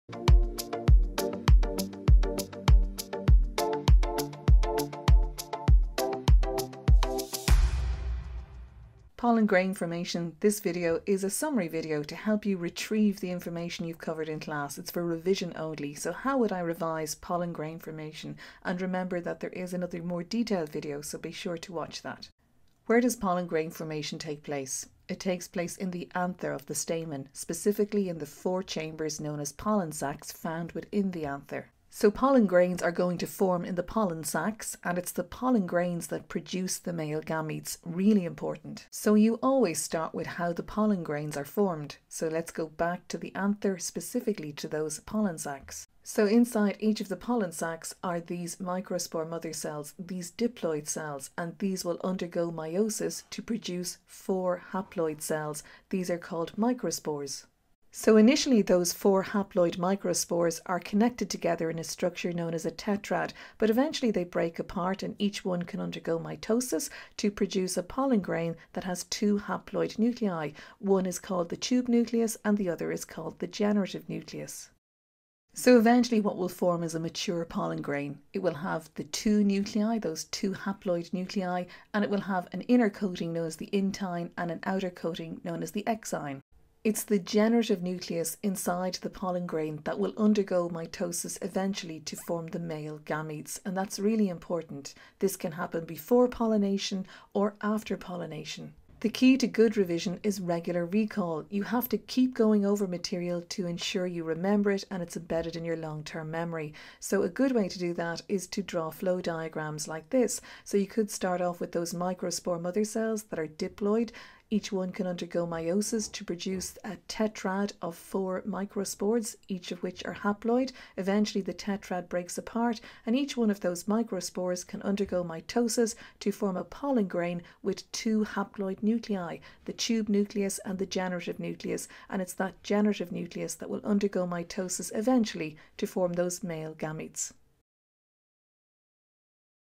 Pollen Grain Formation Video 2A-Revising this topic for leaving cert biology.
A short video to detail a possible approach to assist with leaving cert biology revision of plant sexual reproduction.